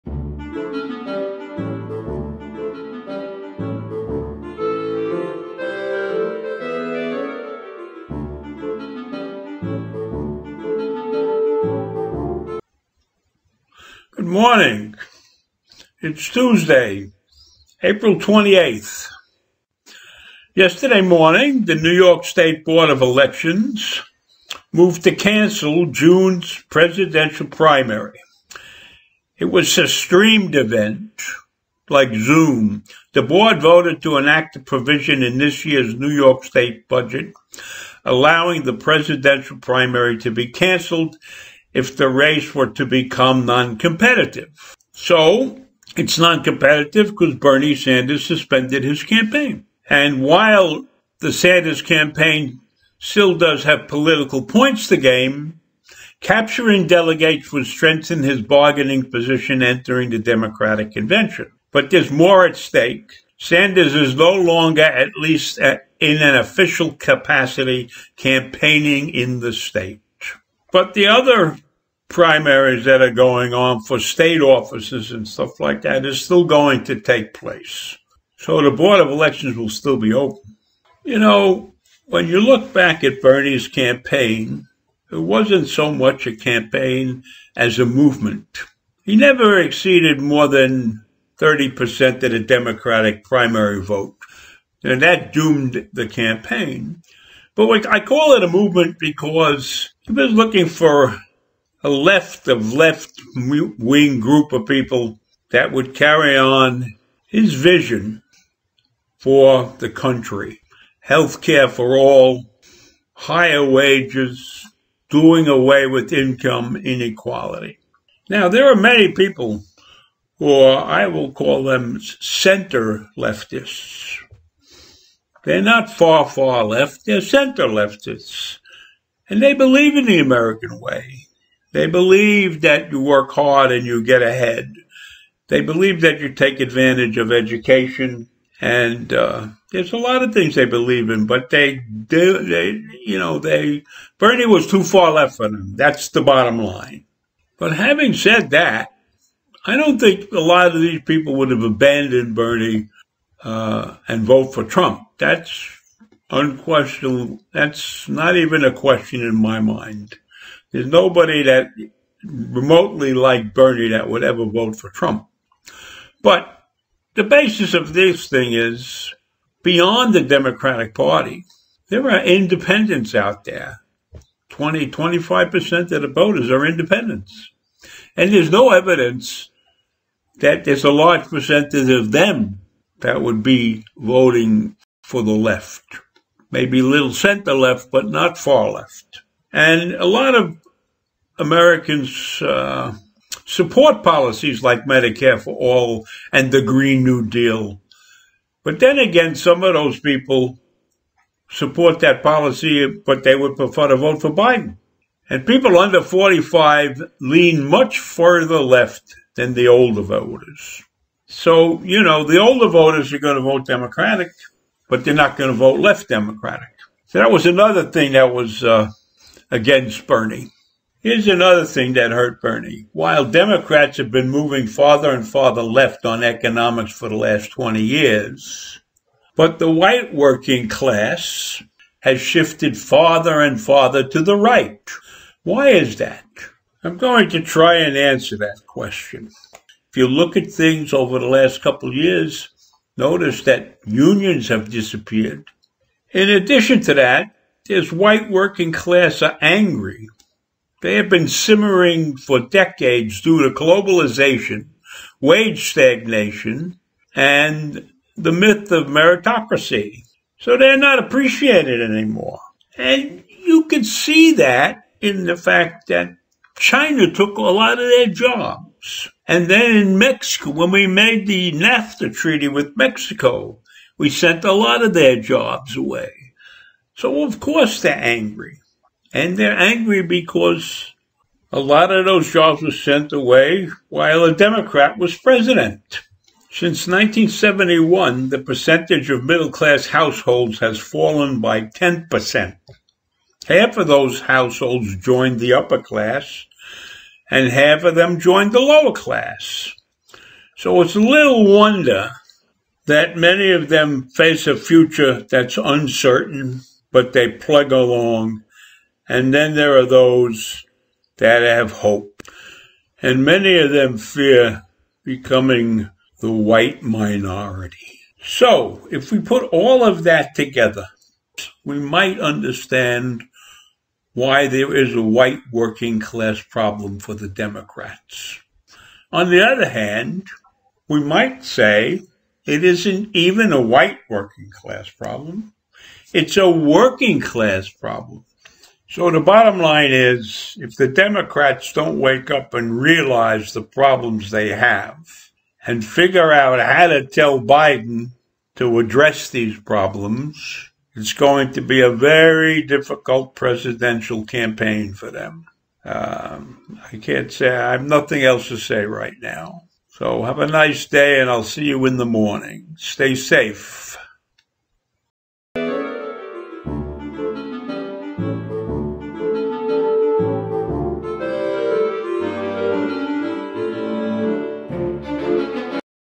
the Democratic National Party cancelled the NYS presidential primary.
Bernie Sanders is pissed bescause he suspended his campaign thus allowing the primary to be cancelled.